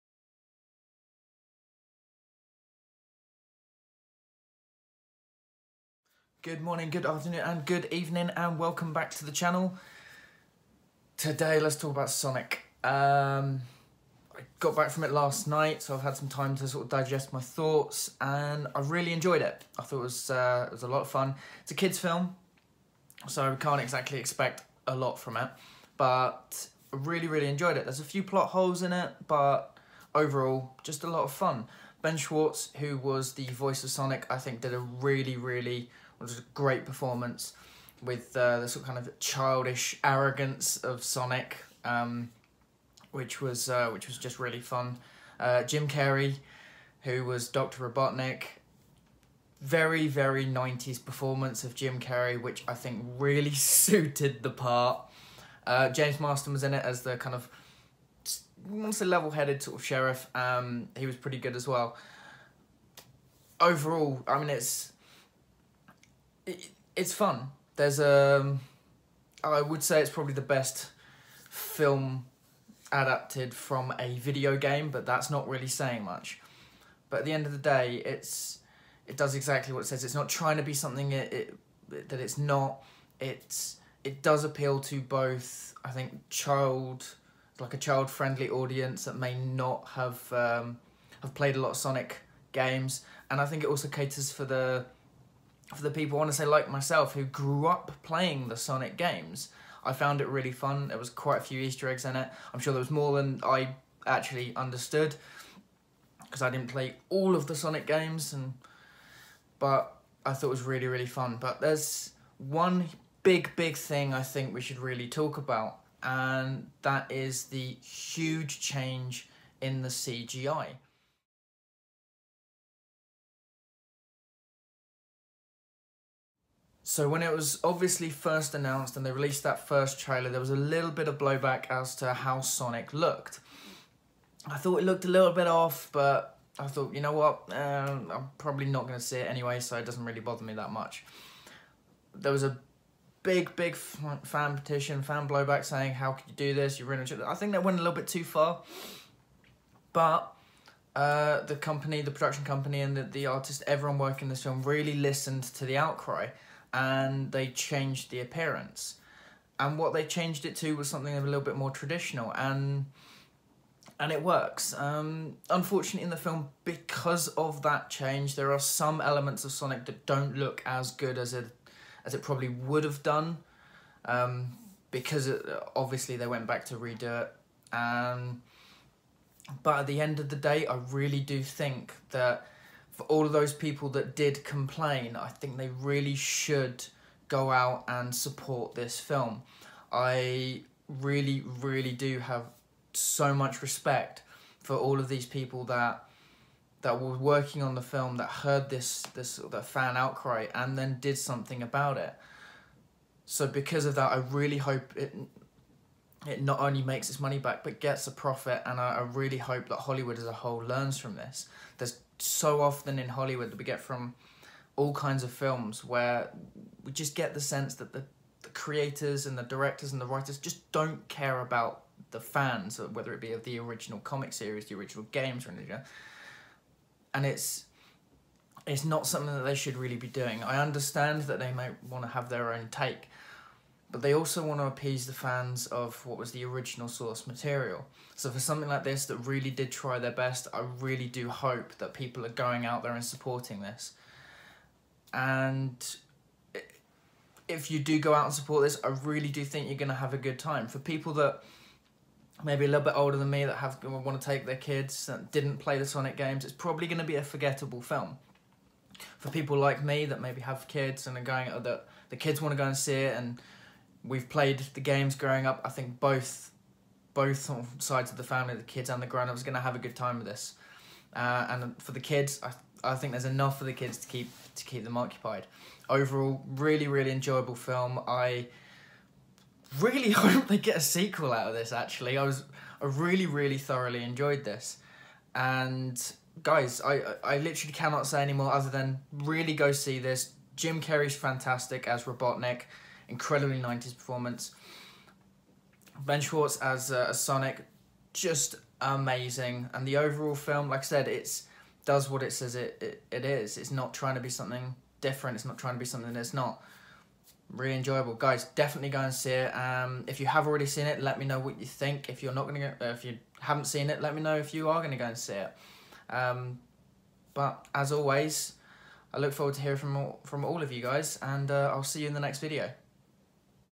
Sonic film review
My review of the 2020 sonic the hedgehog film
Featuring Jim Carrey